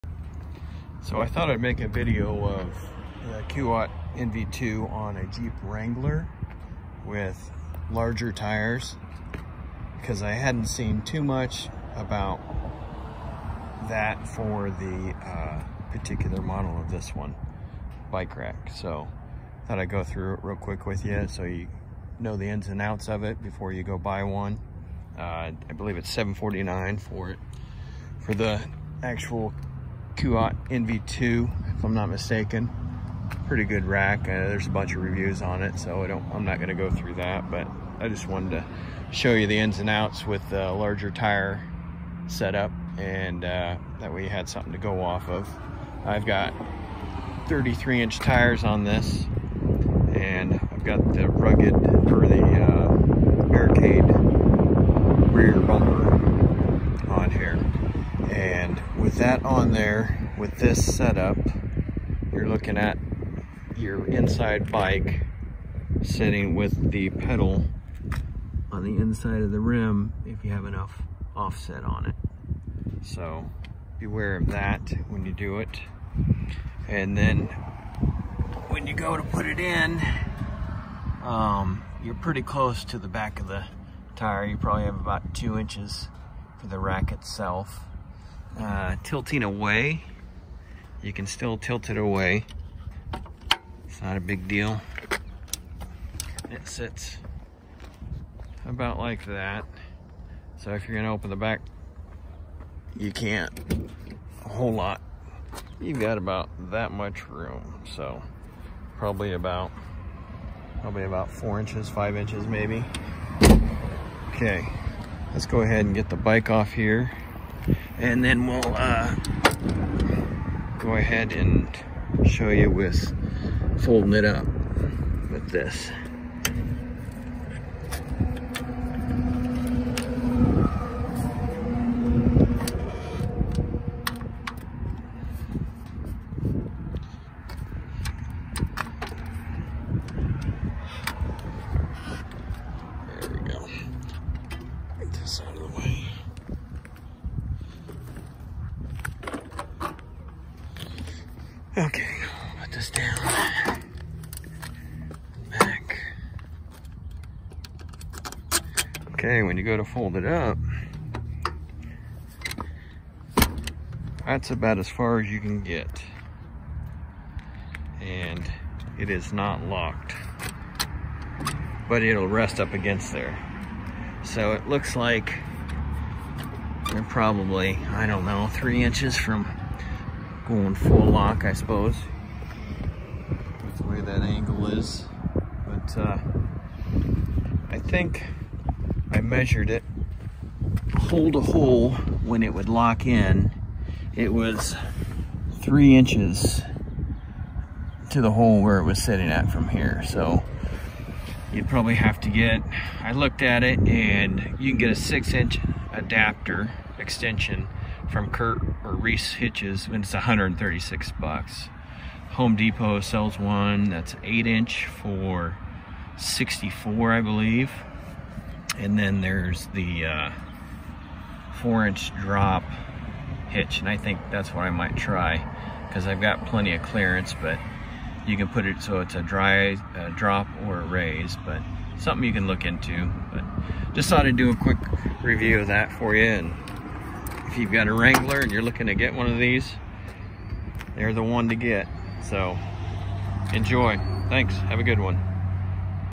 Kuat NV 2.0 Hitch Rack on lifted Jeep Wrangler JK with 33,s review
With larger tires on Wranglers it make it harder to find the right platform rack to put on the back.